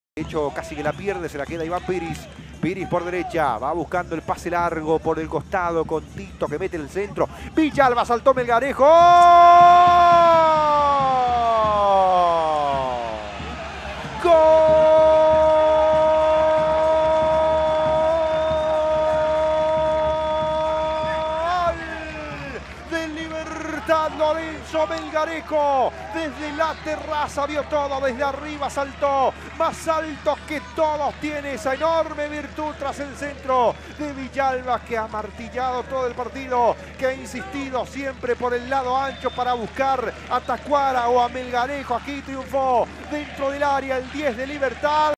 🔥Resistencia 0 vs 1 Libertad 🔥 | Lorenzo Melgarejo a los 56' abre la cuenta para el Gumarelo
De cabeza Lorenzo Melgarejo abrió la cuenta para el Guma en Villa Elisa.